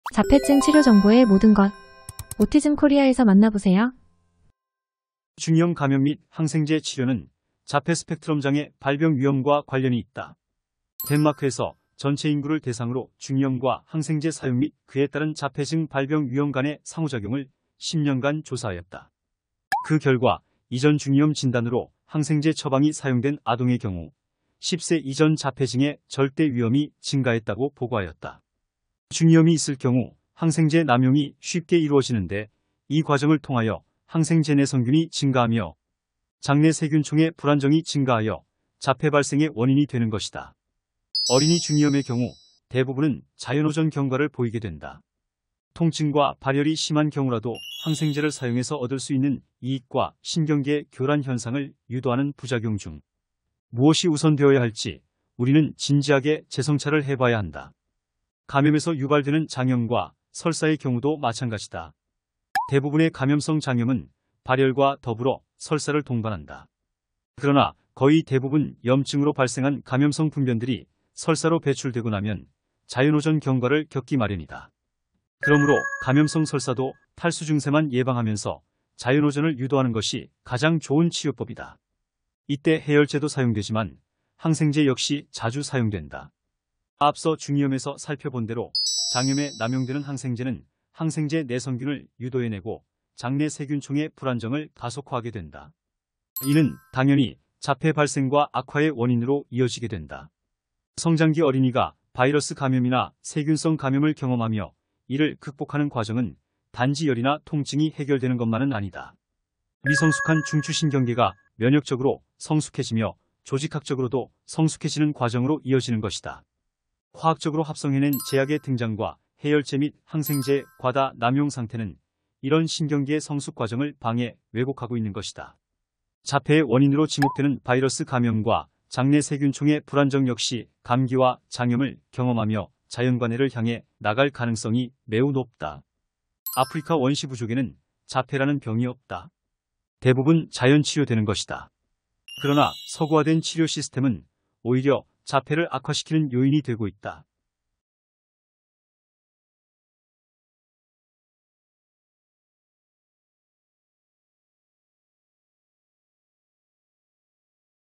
감기와 자폐 4 - 장염, 중이염 치료도 자폐증 악화 요인 중 하나!
읽어주는 닥터토마토 칼럼 : 감기와 자폐 4
장염, 중이염 치료도 자폐증 악화 요인 중 하나!

자폐증 치료 정보의 모든 것

autism korea 에서 만나보세요.

중이염 감염 및 항생제 치료는 자폐스펙트럼장애 발병 위험과 관련이 있다.

덴마크에서 전체 인구를 대상으로 중이염과 항생제 사용 및 그에 따른 자폐증 발병 위험 간의 상호 작용을 10년간 조사하였다.

그 결과 이전 중이염 진단으로 항생제 처방이 사용된 아동의 경우

10세 이전 자폐증의 절대 위험이 증가했다고 보고하였다.

중이염이 있을 경우 항생제 남용이 쉽게 이루어지는데, 이 과정을 통하여 항생제내성균이 증가하며

장내세균총의 불안정이 증가하여 자폐 발생의 원인이 되는 것이다.

어린이 중이염의 경우 대부분은 자연호전 경과를 보이게 된다.

통증과 발열이 심한 경우라도 항생제를 사용해서 얻을 수 있는 이익과 신경계의 교란 현상을 유도하는 부작용 중

무엇이 우선되어야 할지 우리는 진지하게 재성찰을 해봐야 한다.

감염에서 유발되는 장염과 설사의 경우도 마찬가지다.

대부분의 감염성 장염은 발열과 더불어 설사를 동반한다.

그러나 거의 대부분 염증으로 발생한 감염성 분변들이 설사로 배출되고 나면 자연호전 경과를 겪기 마련이다.

그러므로 감염성 설사도 탈수증세만 예방하면서 자연호전을 유도하는 것이 가장 좋은 치료법이다.

이때 해열제도 사용되지만 항생제 역시 자주 사용된다.

앞서 중이염에서 살펴 본대로 장염에 남용되는 항생제는 항생제 내성균을 유도해내고 장내세균총의 불안정을 가속화하게 된다.

이는 당연히 자폐 발생과 악화의 원인으로 이어지게 된다.

성장기 어린이가 바이러스 감염이나 세균성 감염을 경험하며 이를 극복하는 과정은 단지 열이나 통증이 해결되는 것만은 아니다.

미성숙한 중추신경계가 면역적으로 성숙해지며 조직학적으로도 성숙해지는 과정으로 이어지는 것이다.

화학적으로 합성해낸 제약의 등장과 해열제 및 항생제의 과다 남용 상태는, 이런 신경계의 성숙 과정을 방해 왜곡하고 있는 것이다.

자폐의 원인으로 지목되는 바이러스 감염과 장내세균총의 불안정 역시 감기와 장염을 경험하며 자연관해를 향해 나갈 가능성이 매우 높다.

아프리카 원시부족에는 자폐라는 병이 없다.

대부분 자연치료되는 것이다.

그러나 서구화된 치료 시스템은 오히려 자폐를 악화시키는 요인이 되고 있다.